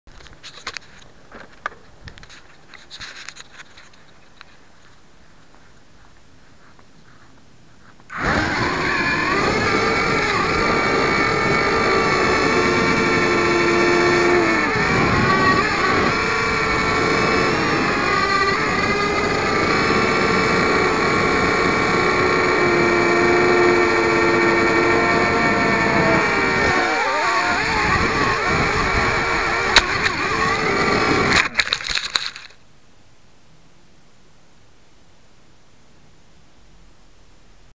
v252 flight with 808 cam
it couldn't do much flying with the camera on but I could hover fairly well for short amounts of time.